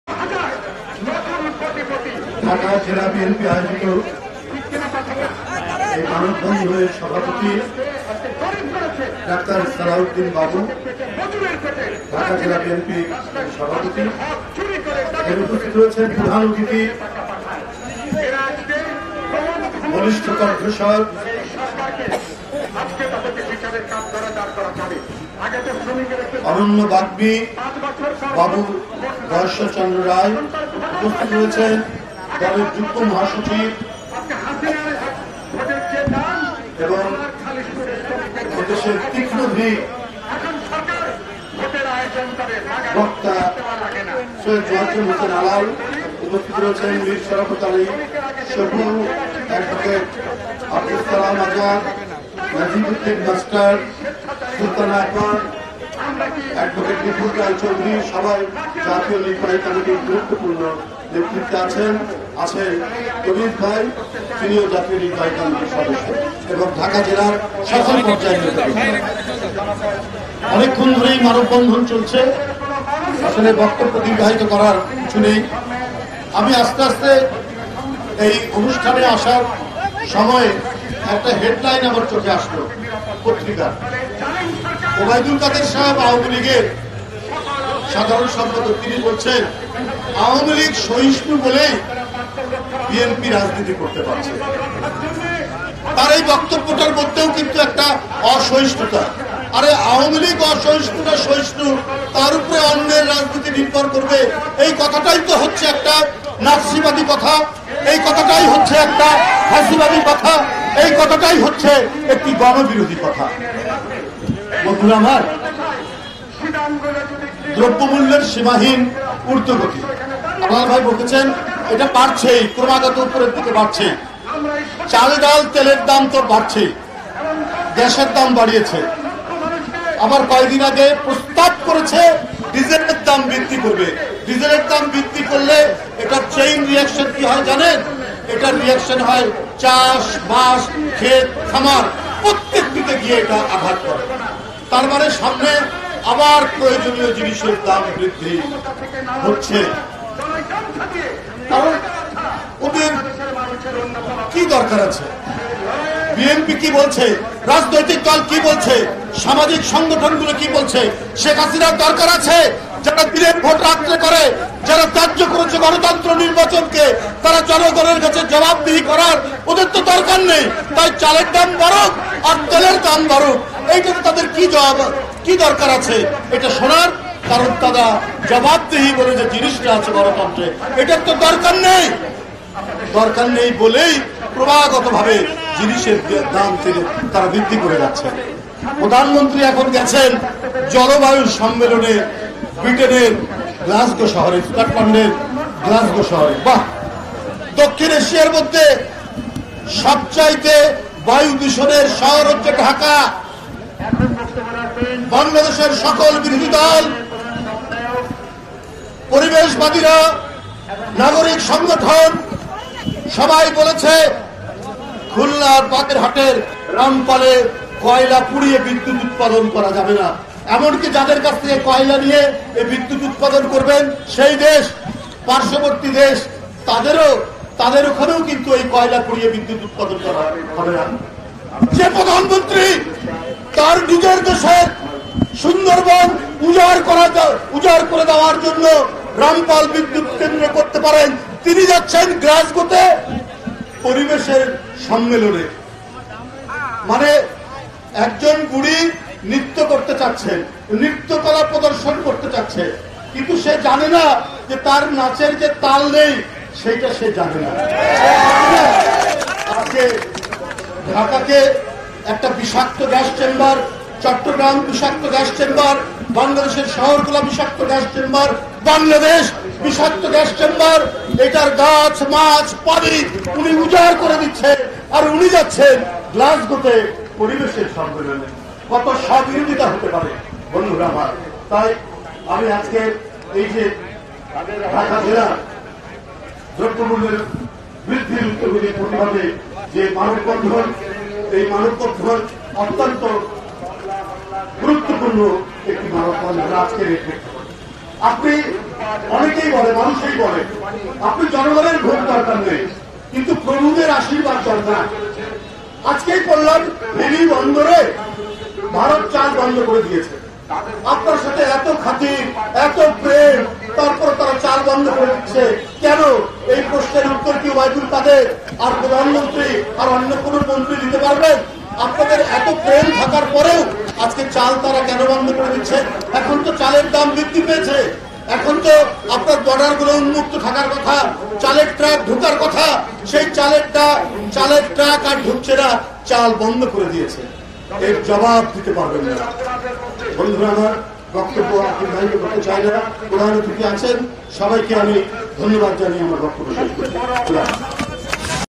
রুহুল কবির রিজভী’র বক্তব্য|| দ্রব্যমূল্য বৃদ্ধির প্রতিবাদে ঢাকা জেলা বিএনপি’র মানববন্ধন
চাল, ডাল, তেল, নুন সহ নিত্য প্রয়োজনীয় দ্রব্য সামগ্রীর সীমাহীন মূল্যবৃদ্ধির প্রতিবাদে আয়োজিত মানববন্ধনে বাংলাদেশ জাতীয়তাবাদী দল-বিএনপি’র সিনিয়র যুগ্ম মহাসচিব এ্যাডভোকেট রুহুল কবির রিজভী এর বক্তব্য।

সোমবার (০১ নভেম্বর), সকালে জাতীয় প্রেসক্লাবের সামনে ঢাকা জেলা বিএনপি’র উদ্যোগে এ মানববন্ধন অনুষ্ঠিত হয়।